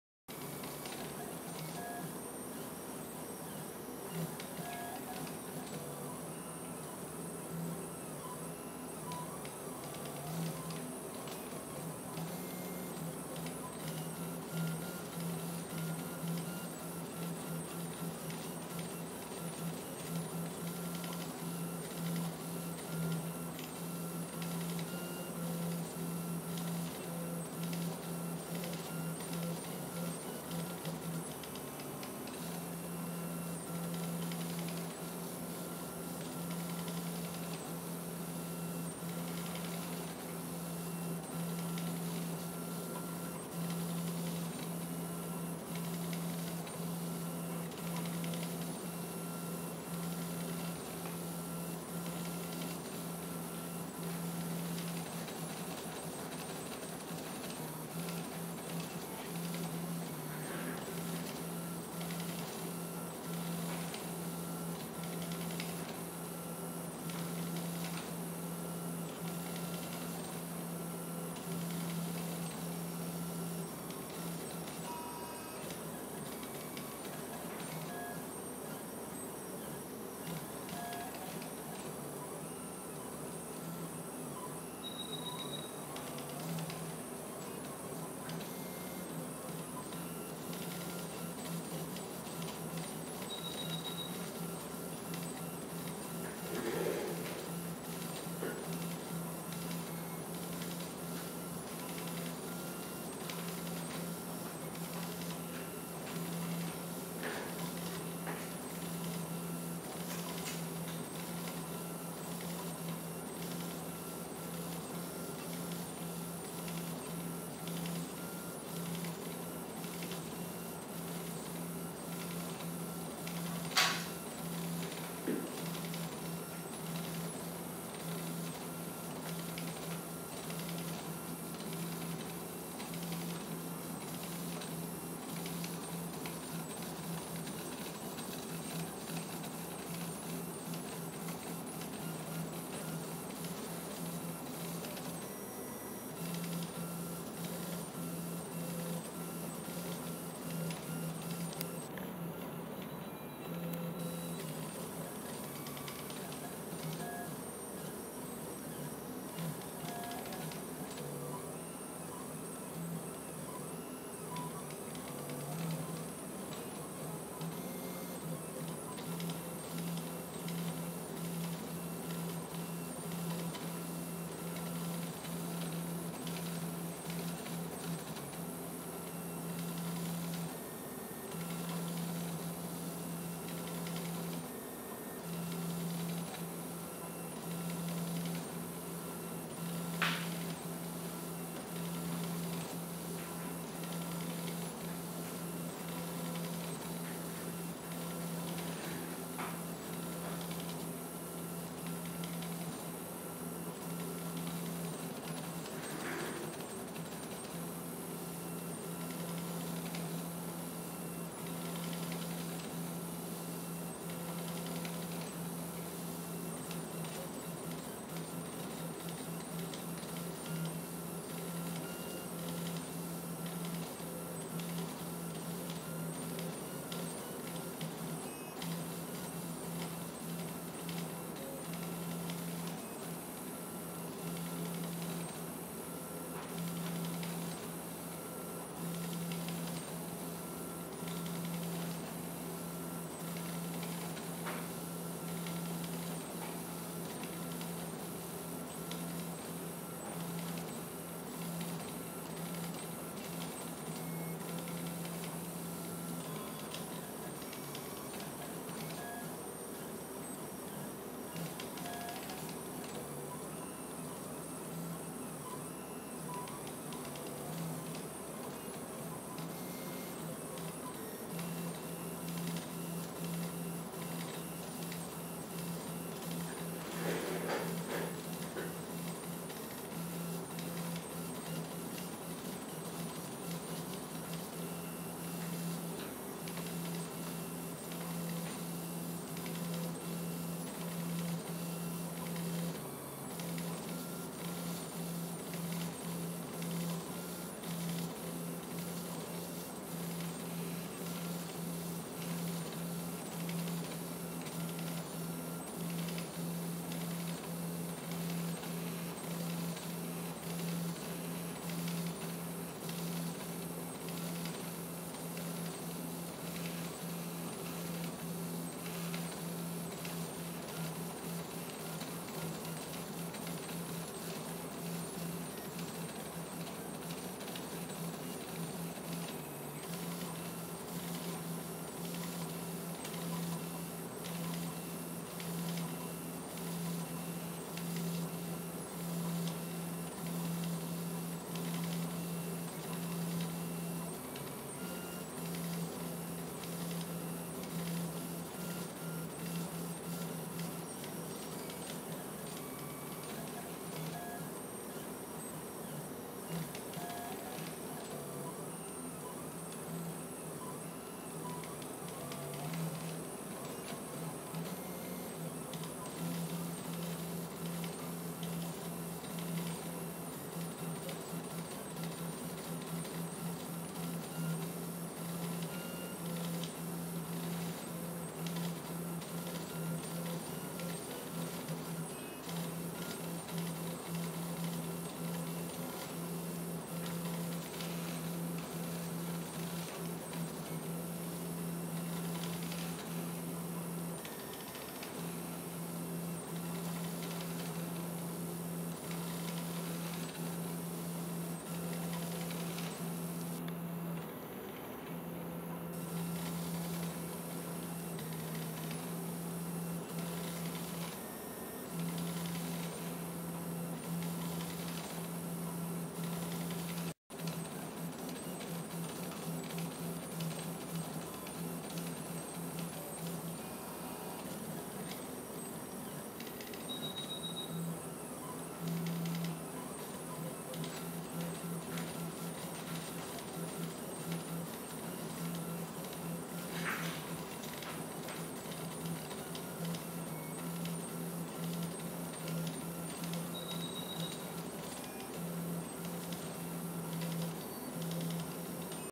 printkbot first print (uncalibrated)
was just too excited to try printing with this printkbot! this print ended up x & y correct and z wrong.